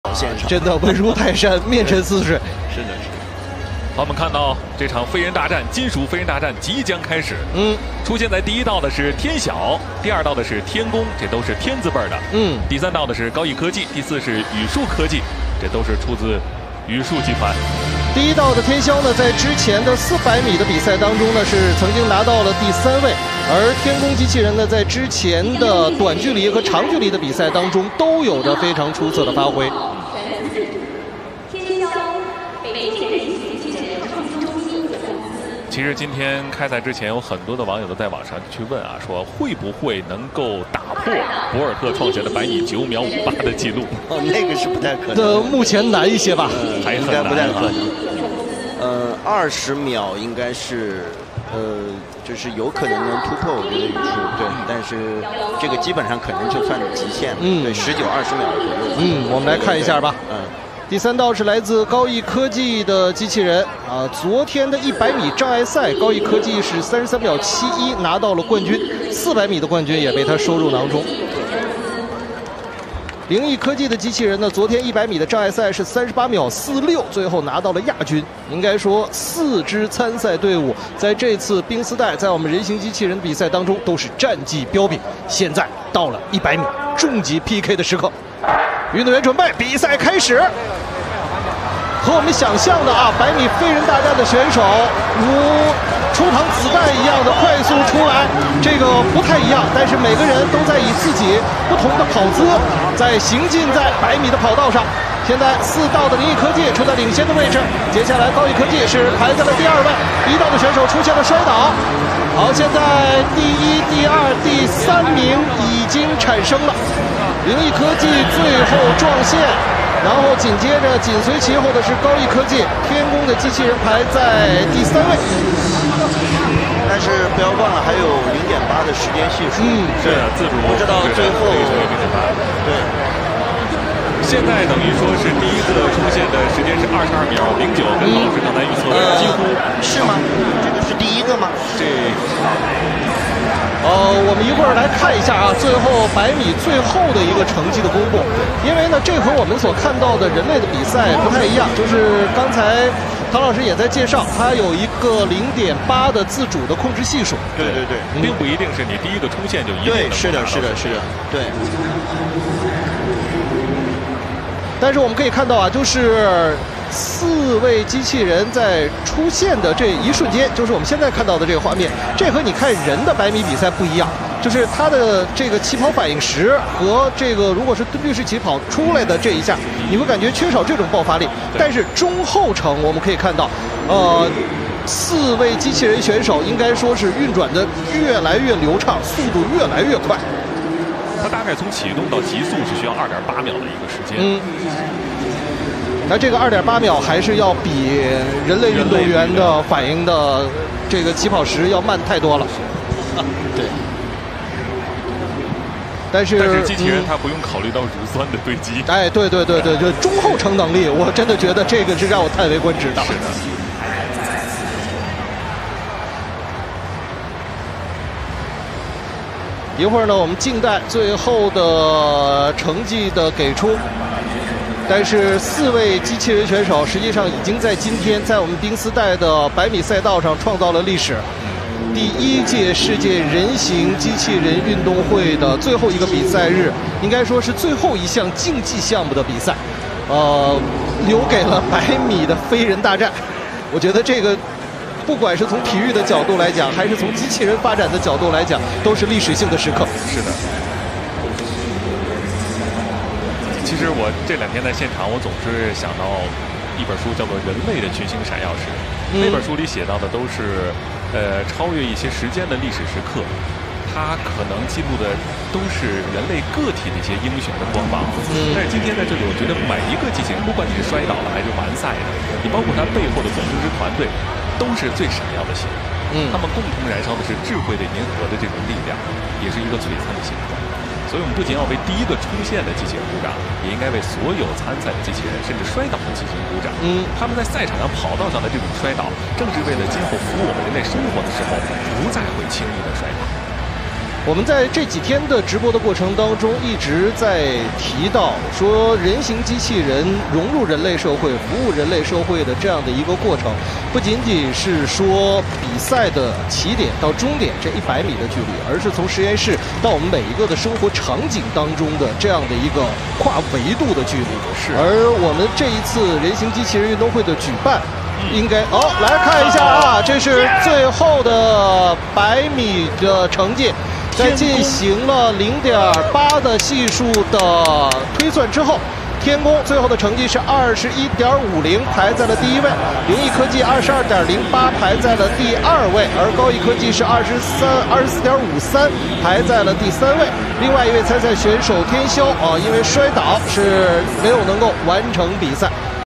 (Eng subs)2025 World Humanoid Robot Games 100m Final,Tian Gong Won!humanoidrobot
In the just-concluded men's 100m "Flying Man" final at the world's first Humanoid Robot Games, "Jushen Tiangong Ultra" from the Beijing Tiangong Team showed strong performance, finally sprinting to the finish line in 21.50 seconds and winning the championship of this event. It is also the first 100m sprint champion in the history of the Games, attracting industry attention to the athletic performance of humanoid robots.